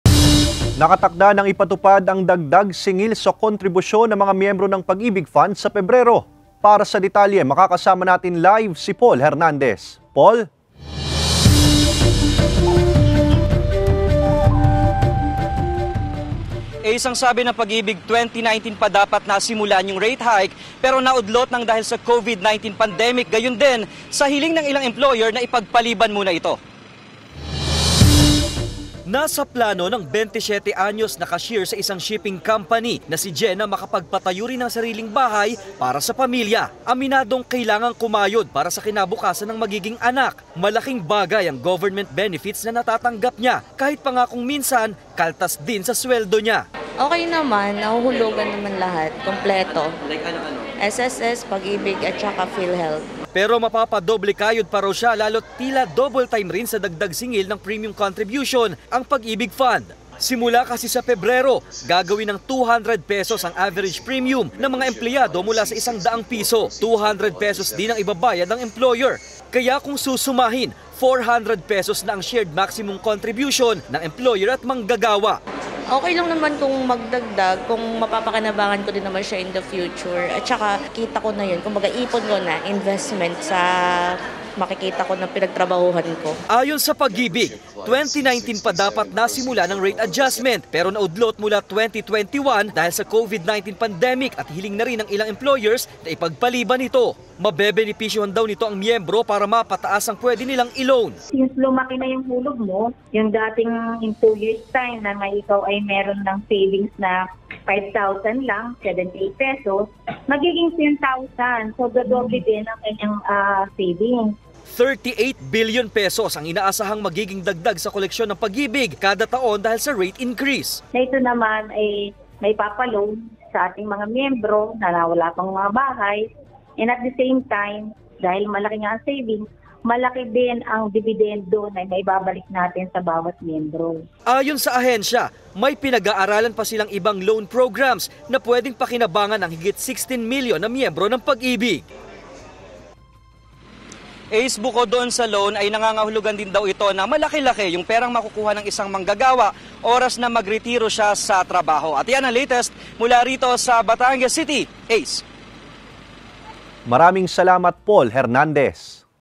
Balitang Southern Tagalog: Dagdag-singil sa kontribusyon sa Pag-IBIG fund, nakatakdang ipatupad
Dagdag-singil sa kontribusyon ng mga miyembro ng pag-IBIG fund, nakatakda nang ipatupad sa Pebrero; Ilang miyembro ng pag-IBIG fund, pabor sa ipatutupad na dagdag-singil sa kontribusyon.